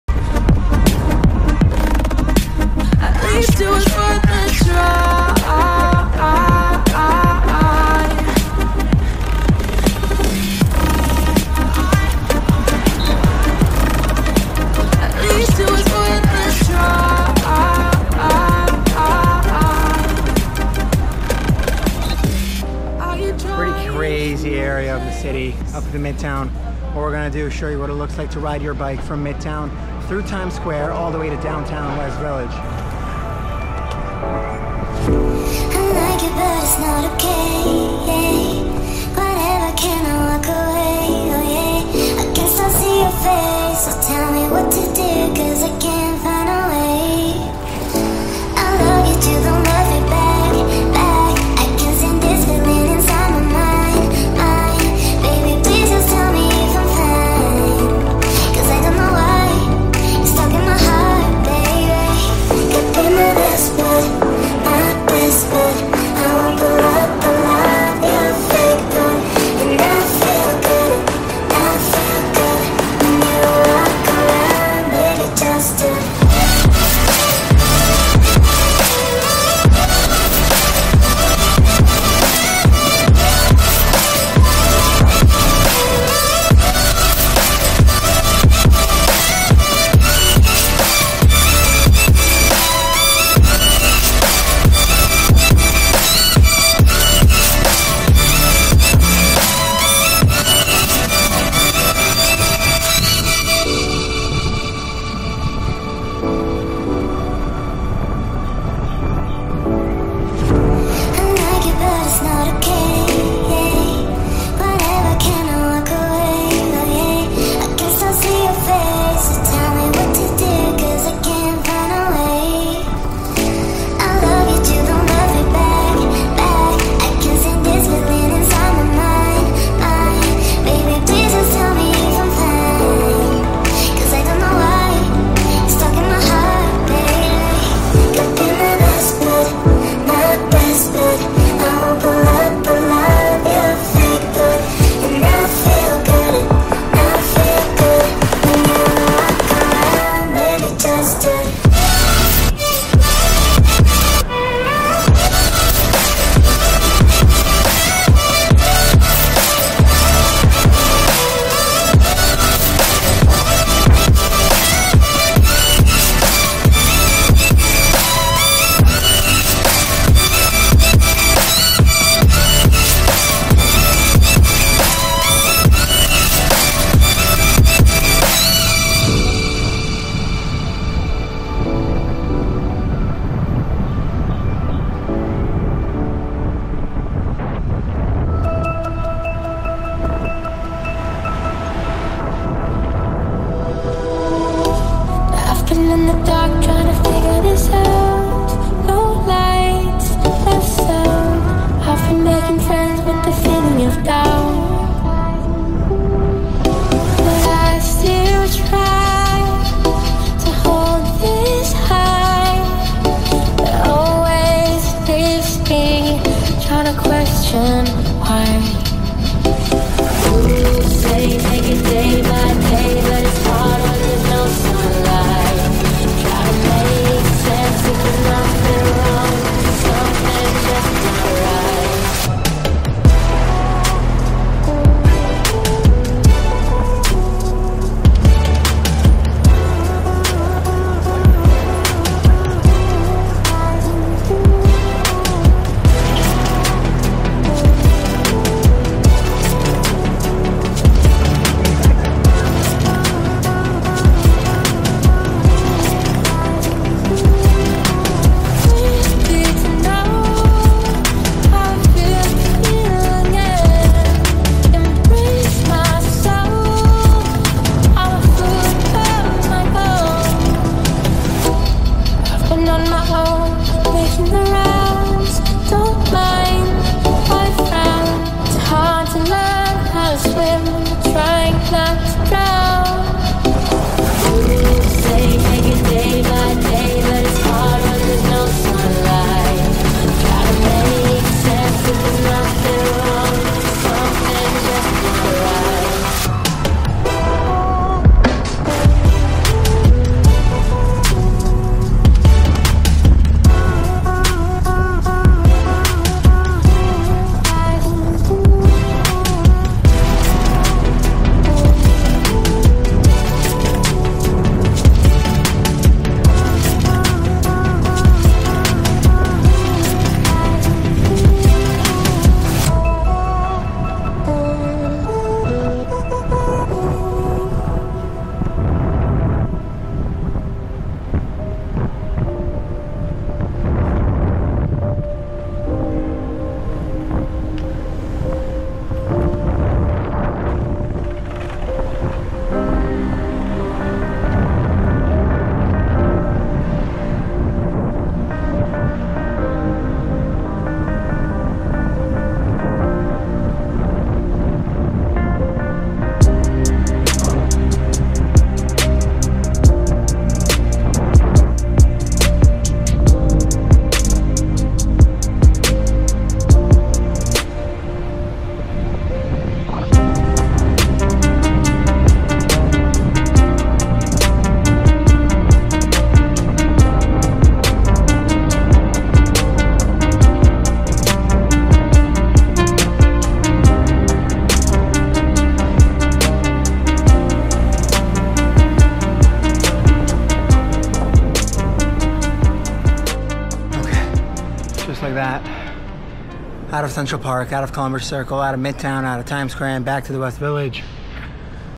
NYC Bike Ride in New York City Central Park, Times Square & Downtown Manhattan!
Join Me for a VO2 Max Ride! 🚴‍♂️
Come along as I tackle a VO2 max ride to stay fit and ready for my next mountain adventure! This time, I’m experimenting with a new camera, capturing great footage, and fine-tuning my editing skills.

🎵 Music Credits